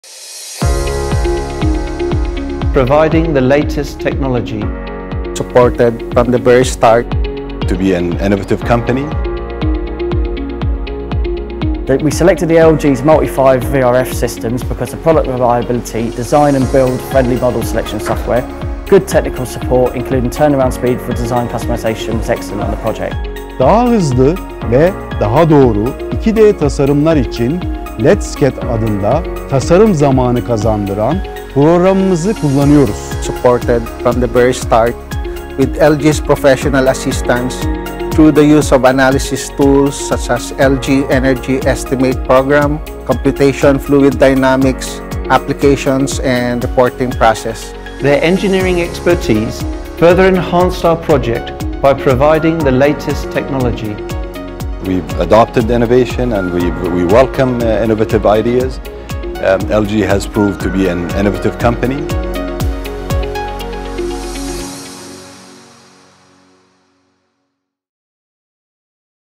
LG HVAC Case Study Compilation_Expertise
LG expertise, technology, and support are helping partners make all the right decisions for installing and operating their VRF systems. Hear straight from our partners how LG is helping their projects succeed.